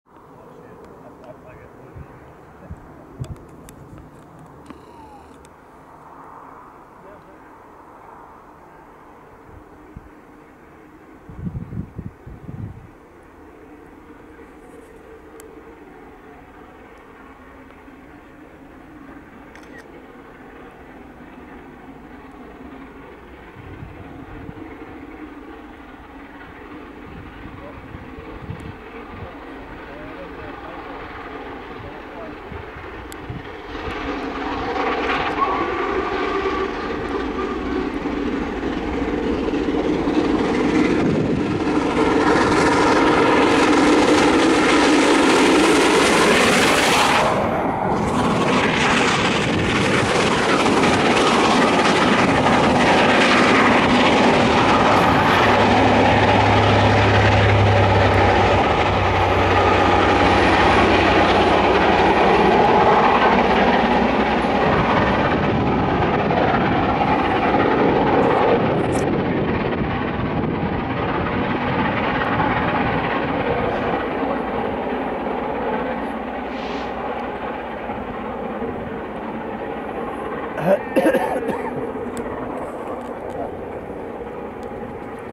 25 Sep 2016 - 46100 Royal Scot south of Gorebridge.
25 Sep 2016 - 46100 Royal Scot climbs the 1 in 87 grade (steepening to 1 in 49) on the Borders Line south of Gorebridge.